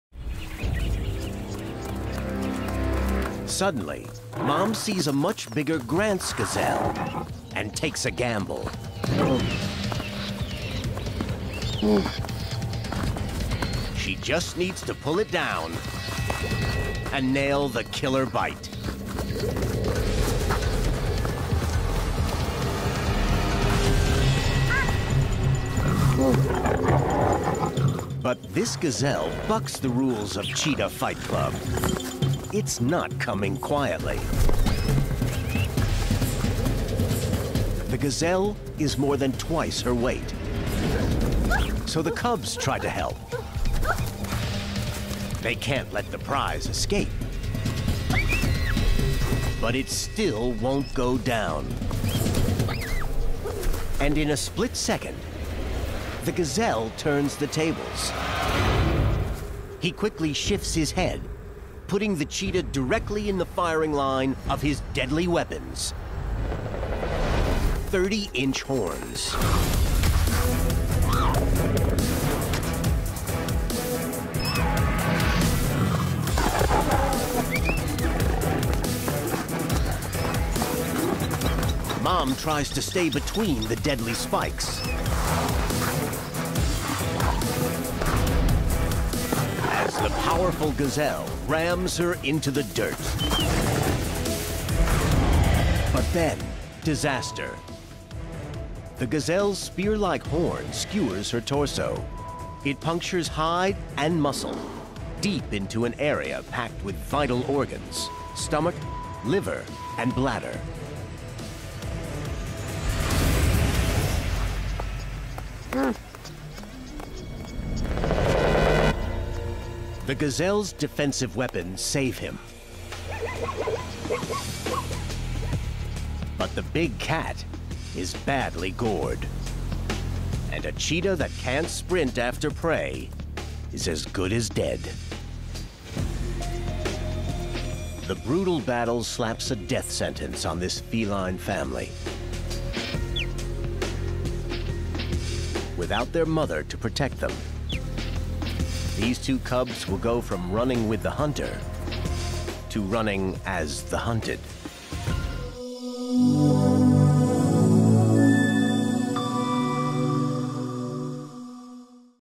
Gazela i gepard
Da li znate koliko gazela može da bude opasna ? Gepard, jedan od najsnažnijih predatora afričke savane, će to upravo iskusiti...